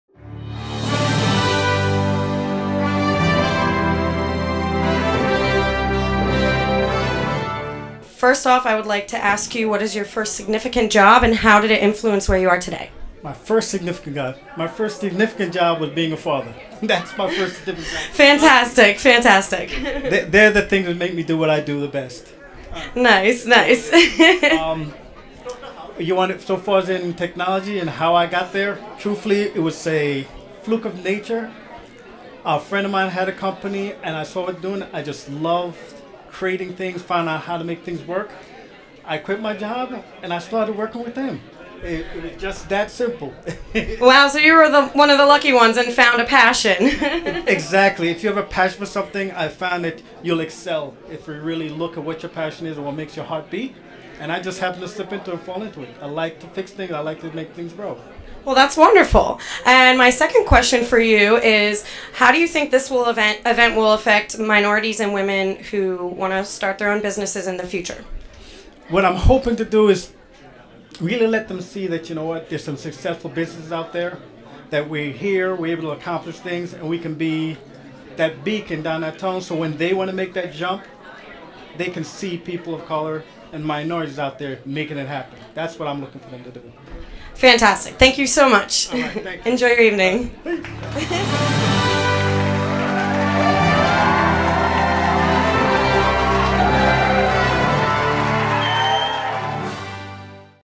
WMBE 5Boro Awards Wayne Roye Interview
This video is about the pre-Awards Ceremony Interview with Wayne Roye, Troinet. Questions: (1) What was your first significant job? and (2) Your take on tonight's event? The 1st WMBE 5BORO Awards Ceremony with Bronx, Brooklyn, Manhattan, Staten Island and Queens Chamber of Commerce on January 22, 2015 at Schimmel Center @ Pace University, NYC. Presented by TD Bank. With Wayne Roye and Ashley Bevensee. Filmed and Edited by Edge City Design, LLC